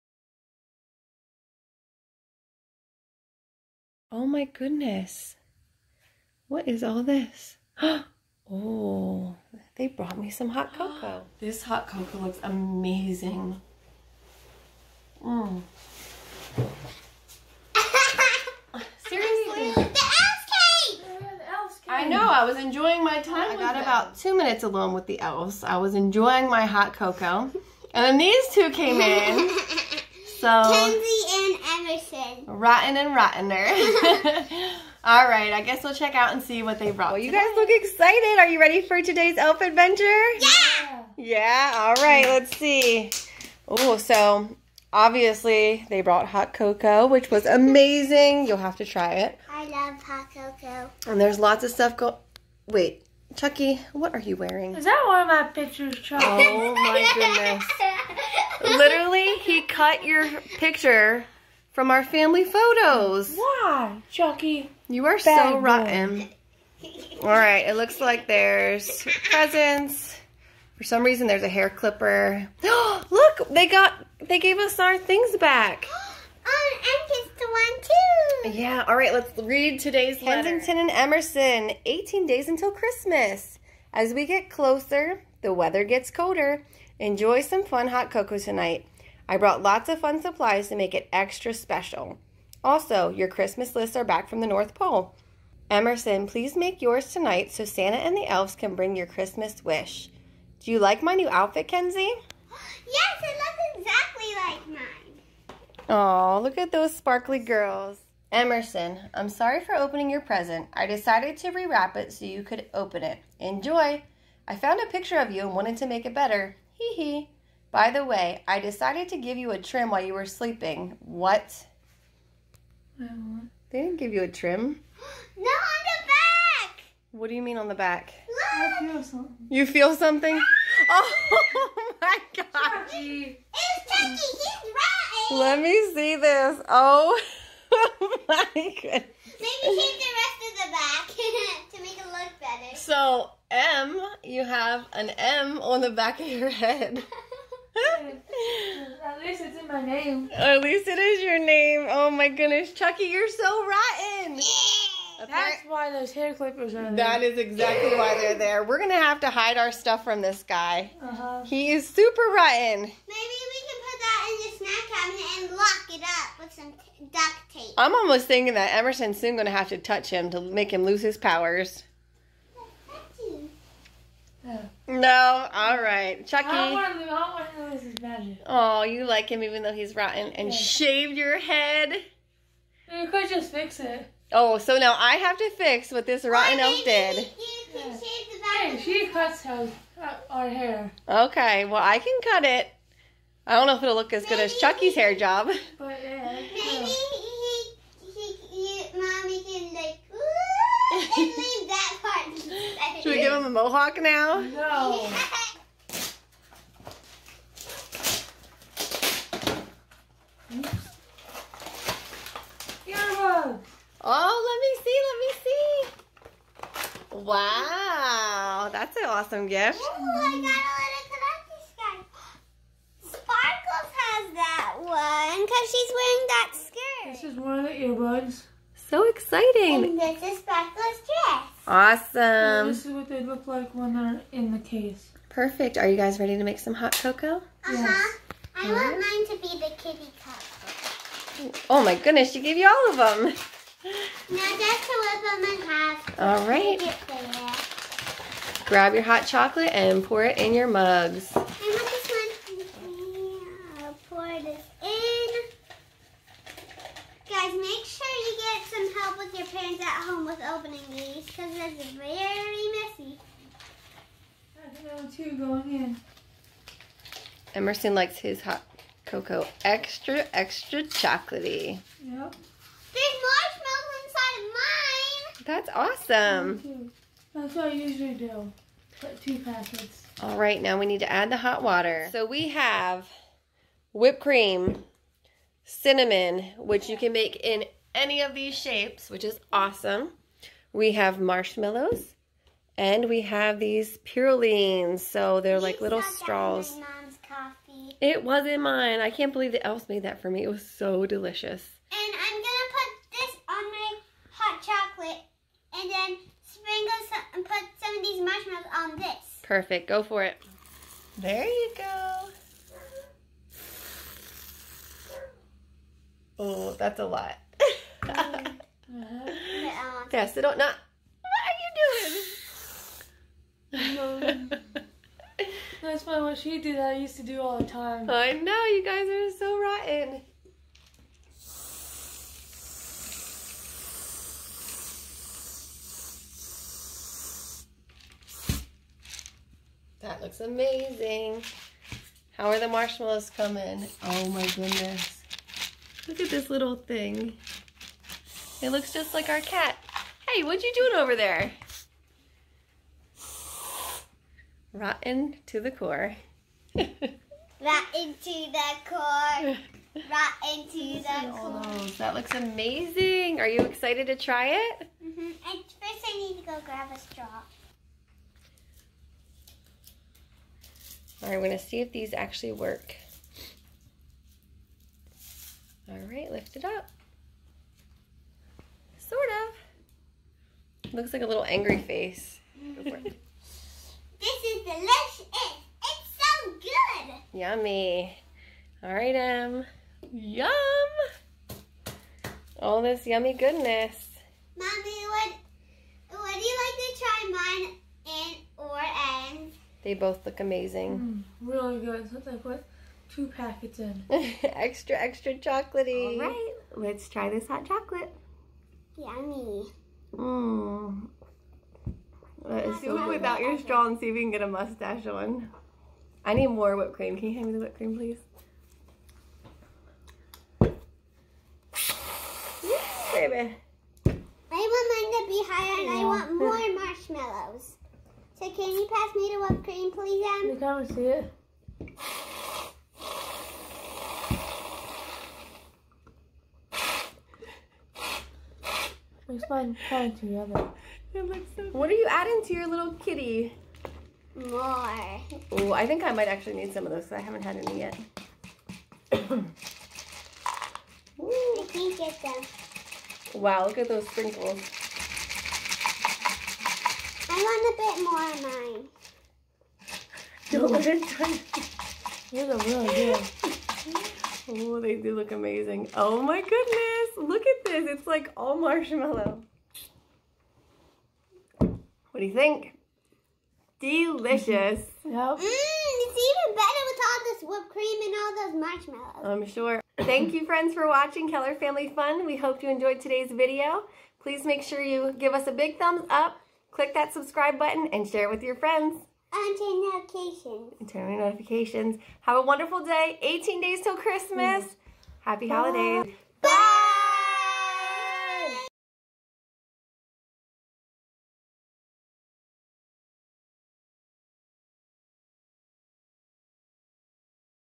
17 Days Until Christmas 🎄 Countdown - Chucky shaves Emerson's Hair - Sparkles Brings Hot Cocoa!!!
Oops Sparkles wrote the wrong date. ONLY 17 days until CHRISTMAS!!!